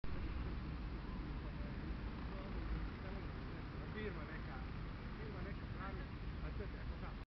Top puppy 2x VA1 Remo vom Fichtenschlag grandson- Ruf Ultra Star with 2,5 months old
Perfect male puppy-Ruf Ultra Star with 2,5 months old-,grandson of 2x VA1 Remo vom Fichtenschlag,father: SG35 BSZS 2015. Baru von Marbo Wolf (2x VA1 Remo vom Fichtenschlag) mother Anda Ultra Star (granddaughter of Young World Champion SG1 BSZS Tyson Fixfrutta))